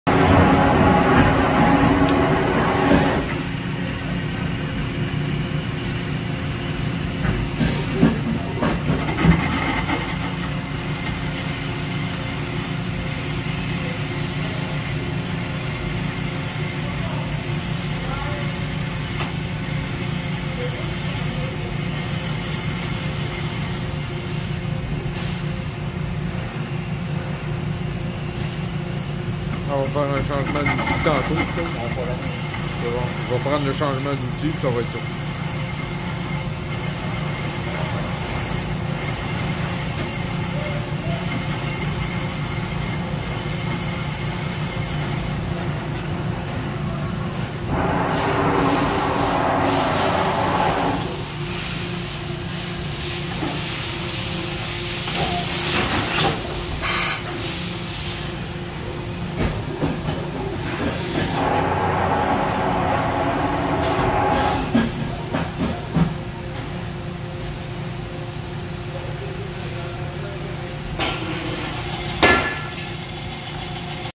2008 MAZAK INTEGREX 300 IV S MULTI-AXIS CNC LATHE - #13344
2008 Mazak Integrex 300 IV S Multi-Axis CNC Lathe, Y-Axis, 12000 RPM Live Tooling, B-Axis, C-Axis Indexing, Dual 10" 3-Jaw Chucks, Parts Catcher, Tool Presetter, Chip Conveyor